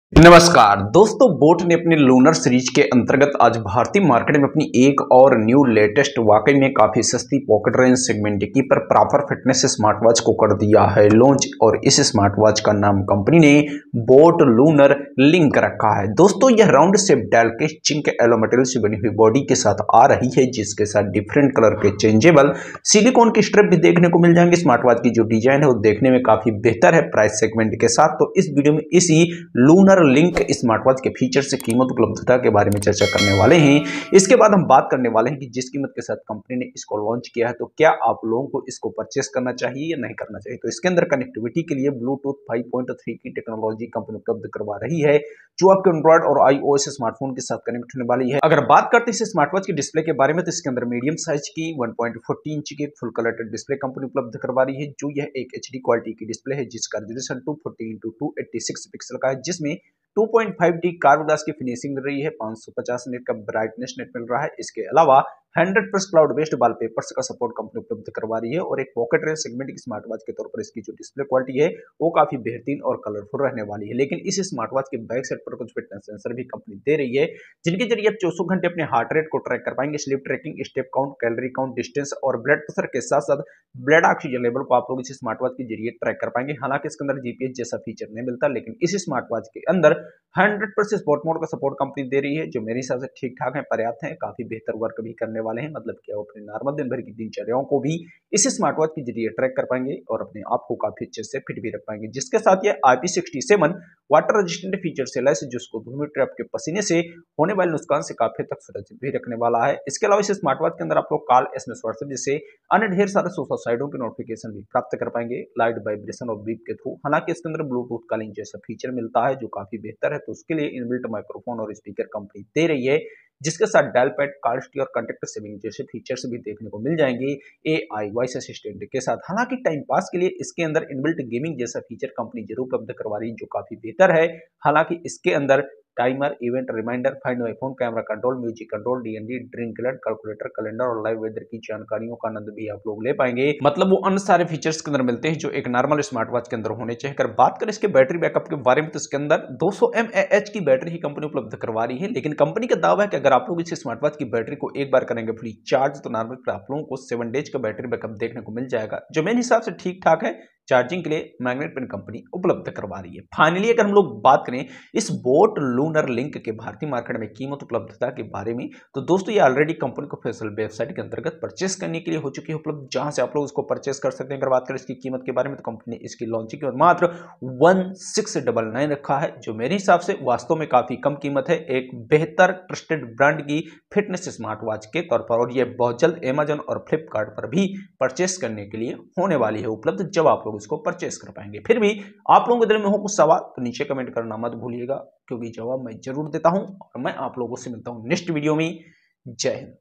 Boat Lunar Link Smartwatch | 1.40" HD Display | BT Calling | In- Built Games | Features | Price
boAt has launched its latest boat lunar link smartwatch in india,

the smart watch has a 1.40-Inch HD Display 2.5D coverd display with 550- britness display that supports over 100+ cloud watch faces that can be set you,

the lunar link has waterproof features IP67 and bluetooth calling features you can enjoy clear hand free calling with voice assistant support in the watch,

the boAt Lunar Link comes with a SpO2, blood oxygen level monitoring. and guided meditative breathing app along with a 24x7 automatic heart rate monitor, it will come with music controls, find my phone, DND, and live weather forecasts,

the boat promises up to 7 days of battery life with normal use and you can enjoy 100+ sports modes.There is indoor walking, outdoor walking, yoga, workout, and more.

the boAt lunar link are yet to be announcing and available via boAt’s official website india and amazon, flipkart price Rs.1,699 /- only